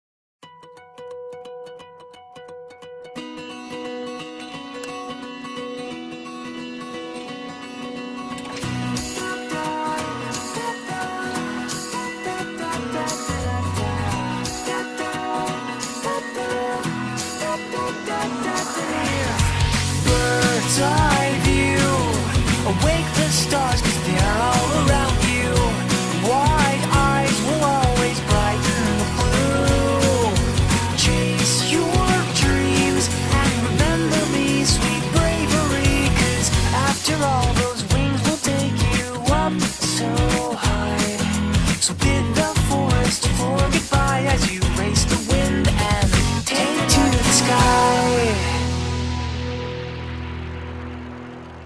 α вяєαтн σƒ мιѕт αη∂ муѕтєяу [HD]
I rode ChiChi and got some clips with my brothers camera!
no critique please[: thanks.